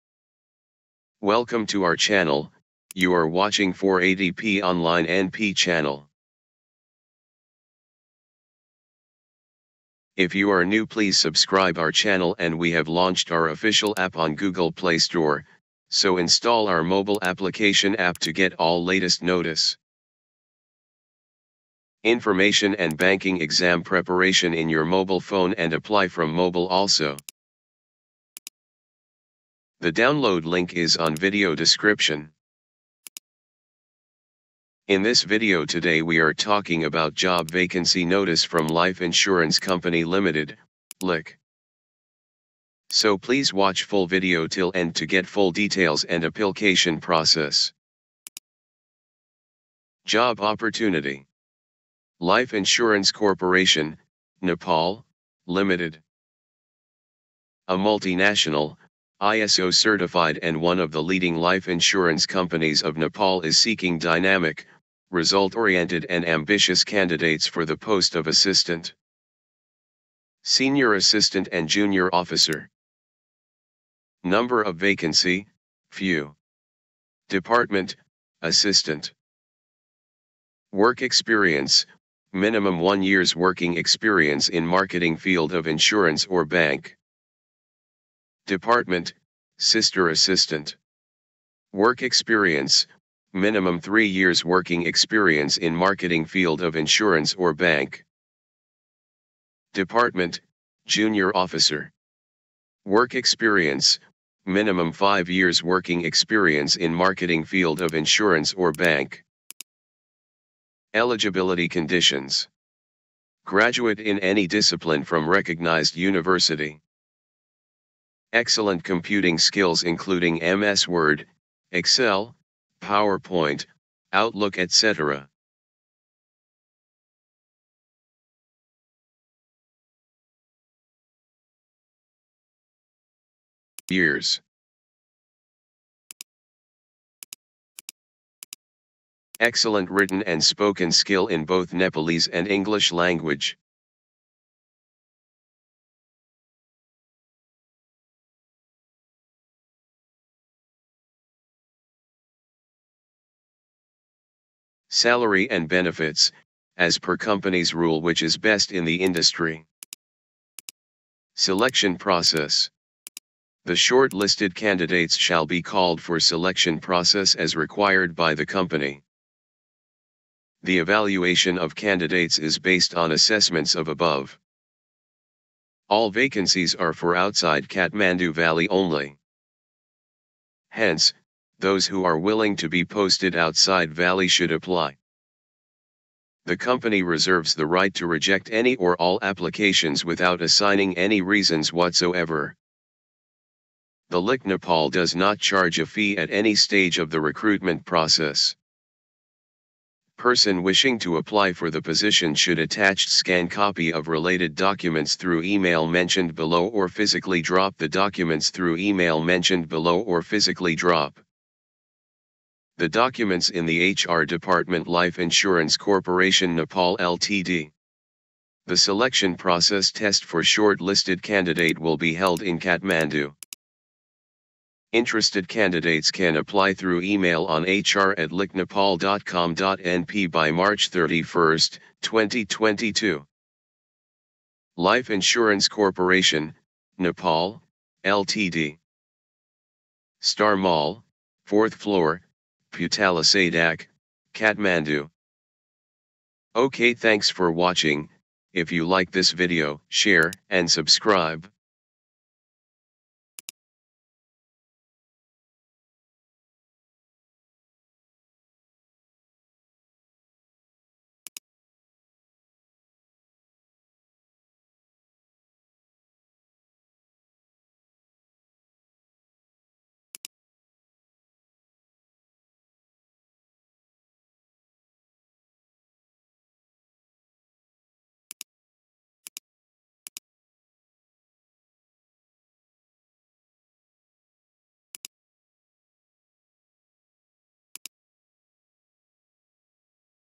Life Insurance Company Ltd. (LIC) मा रोजगारीको अवसर, माग्यो पदमा कर्मचारीहरु - Job Vacancy 2022
Life Insurance Company Ltd. (LIC) मा रोजगारीको अवसर, माग्यो पदमा कर्मचारीहरु - Job Vacancy Notice 2022 - Assistant, Sr. Assistant & Jr. Officer - Qualification: Bachelor's Degree / Form भर्ने सम्पूर्ण तरिका सहित राखिएकोले पुरा भिडियो अवश्य हेर्नुहोला।